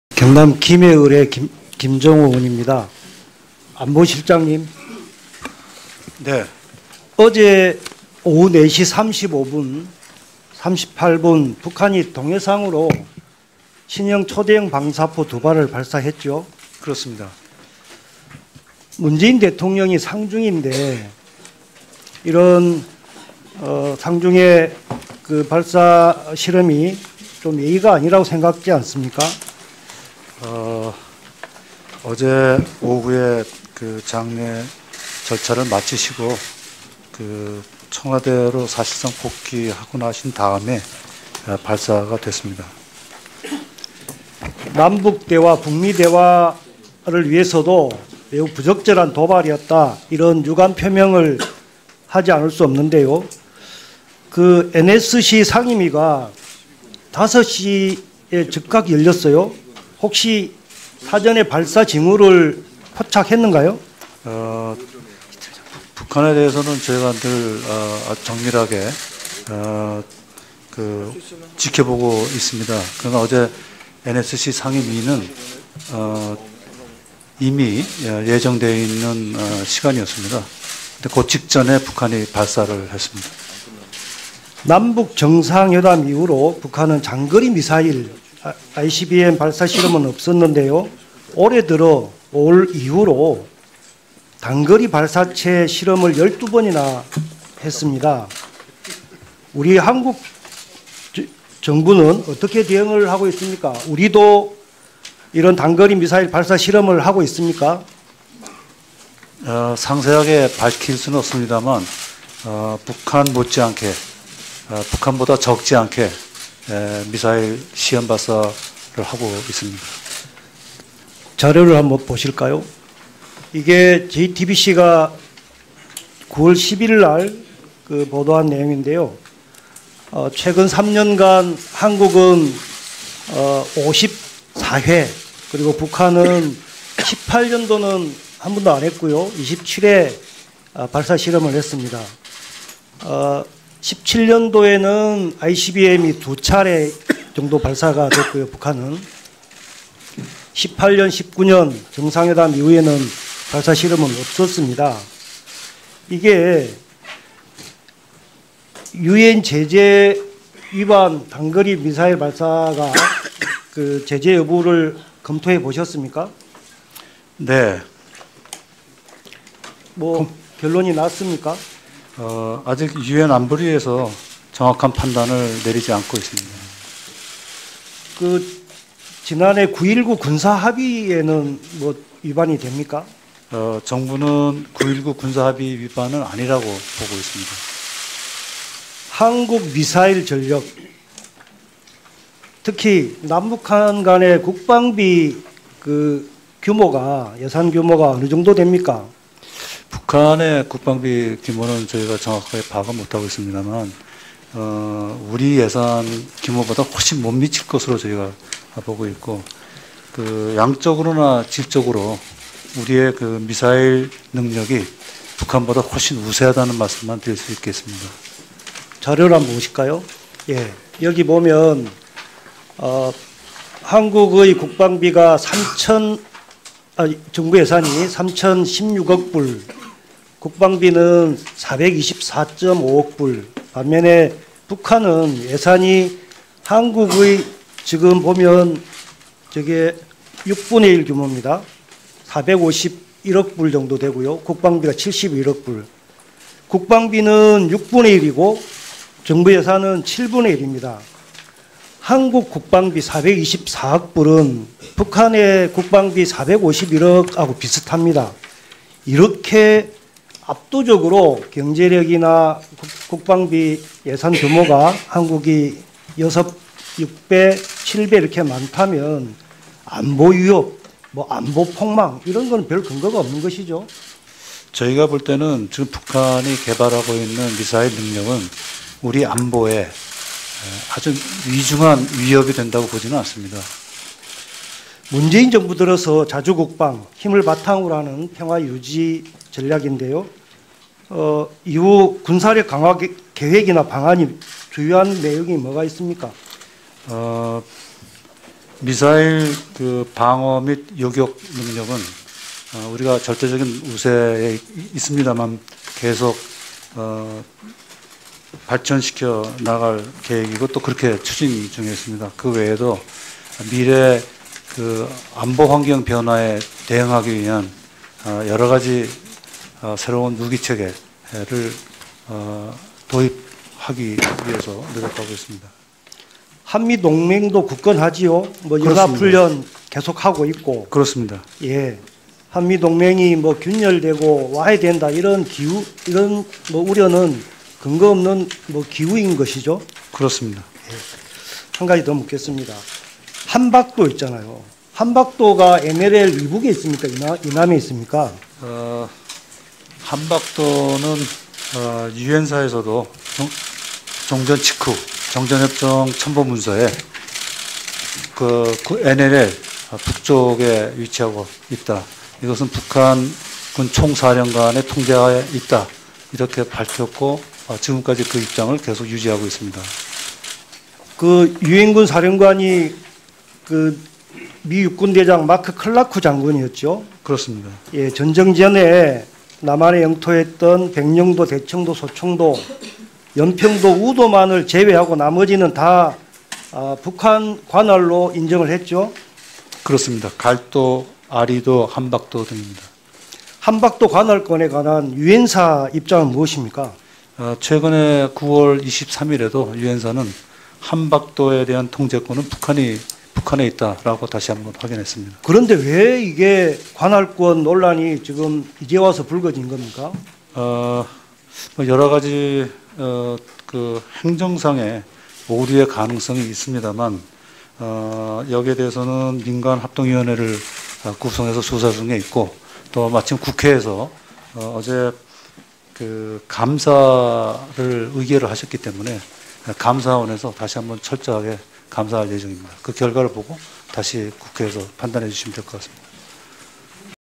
[2019 국정감사] 국가안보실 - "문재인 정부의 안보정책이야말로 성공한 정책"(191101)
2019년 대통령비서실, 국가안보실, 대통령경호처 국정감사
국회의원 김정호-국가안보실장 정의용 질의응답